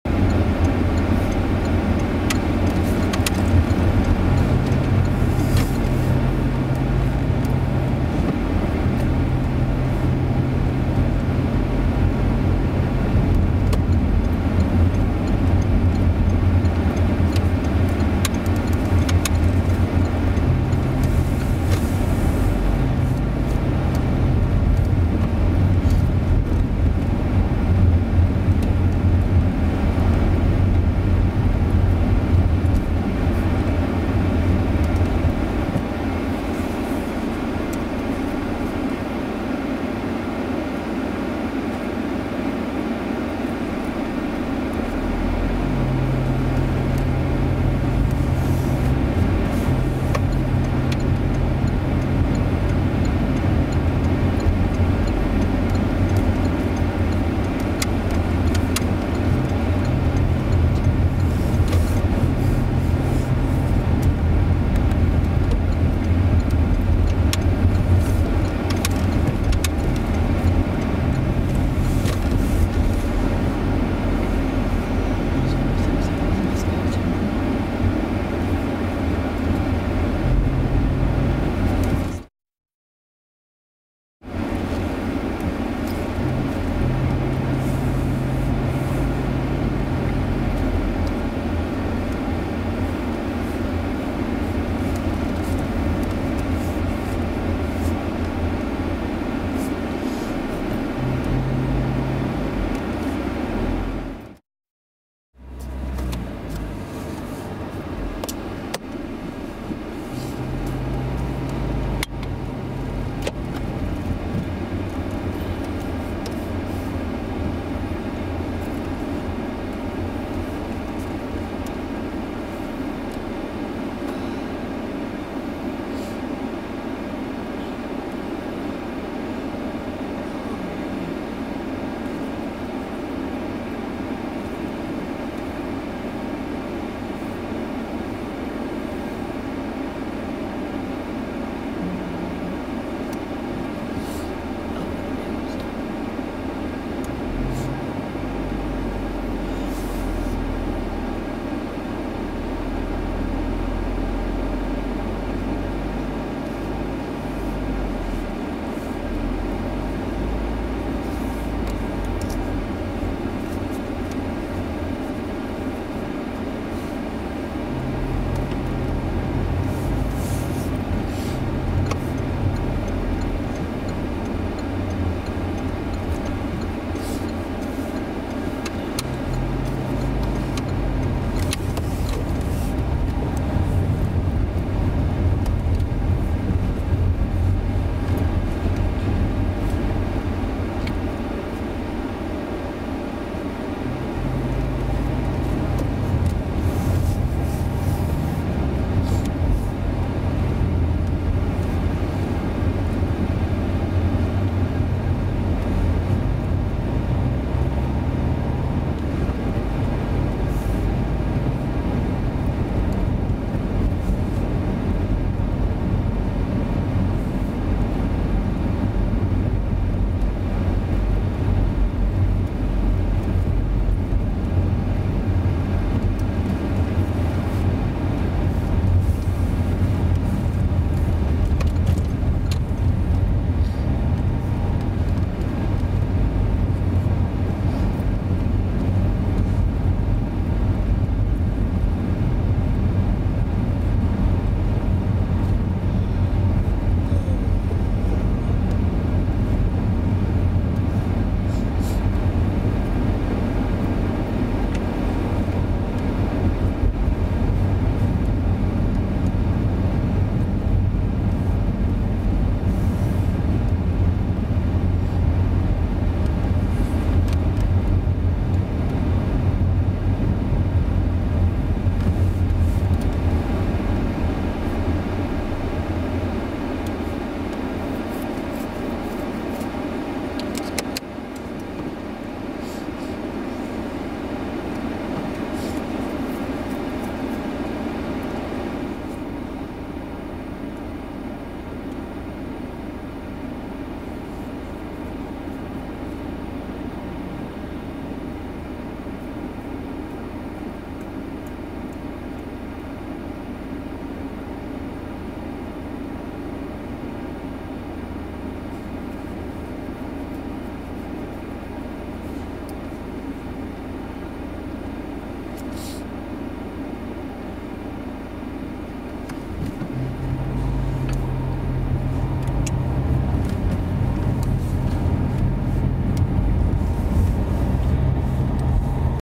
Driving On Foregate Street, Pierpoint Street, Sansome Walk & City Walls Road, Worcester UK
Driving along Foregate Street A38, Pierpoint Street, Sansome Walk A38 & Sansome Place (with a brief stop at St. George's church car park), Sansome Place, Sansome Walk A38, Sansome Street A38 & City Walls Road A38, Worcester, Worcestershire, England
Videoed on Sunday, 2nd August 2015